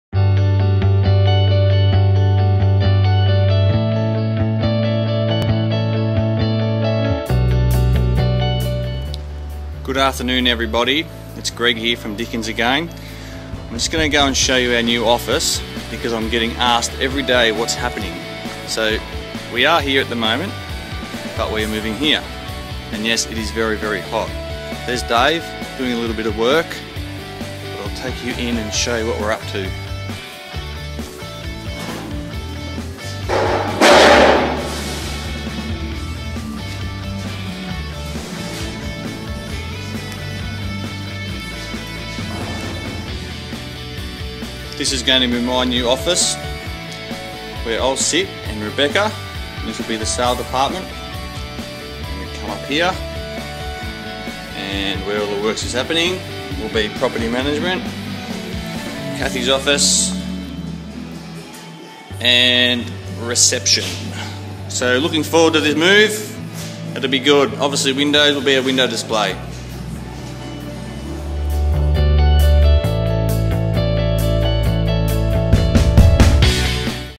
What's happening at 99 Myrtle Street, Myrtleford?
There has been lots of activity happening at 99 Myrtle Street and naturally we have received lots of enquiries about what is happening next door. It's time to find out... Our sales representative Greg Doodewaard drops by to check out the progress of our new Myrtleford office.